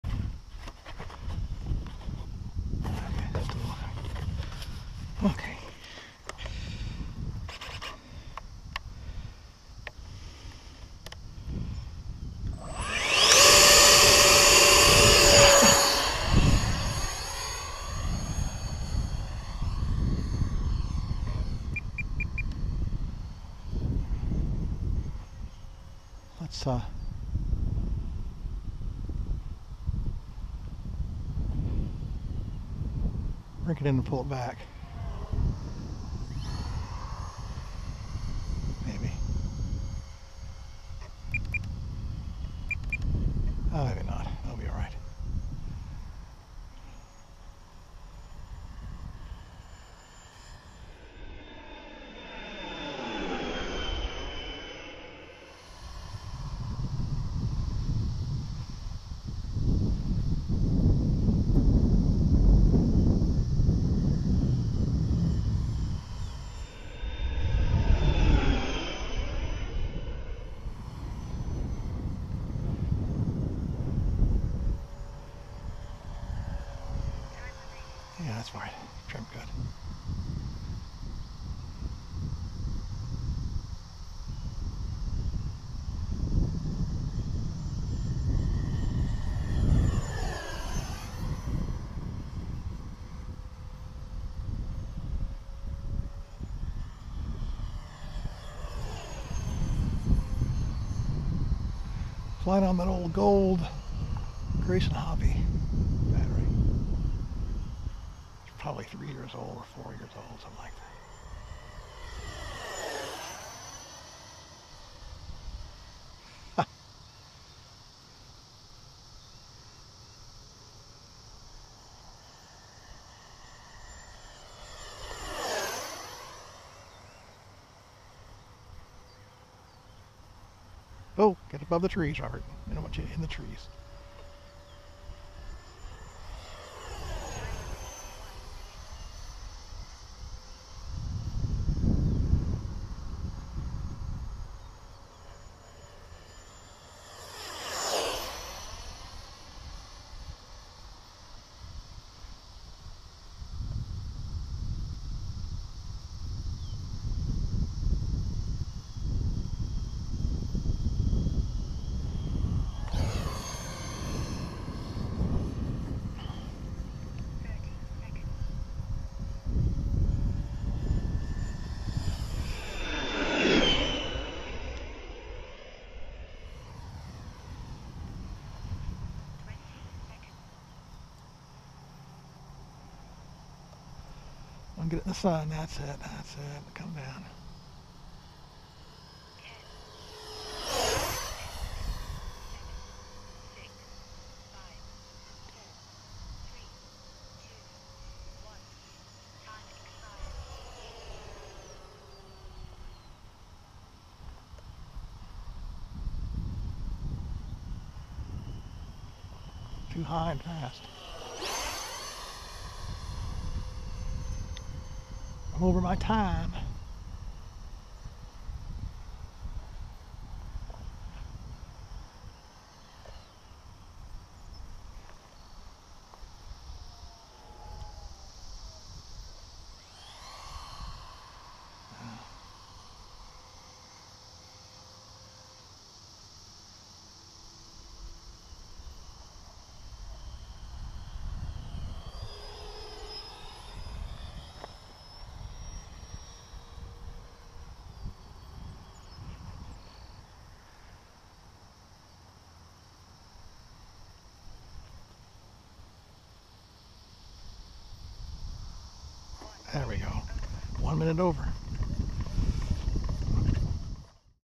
SUPER HORNET SUNDAY
My Freewing 64mm F-18E SUPER HORNET out for a SUNDAY spin.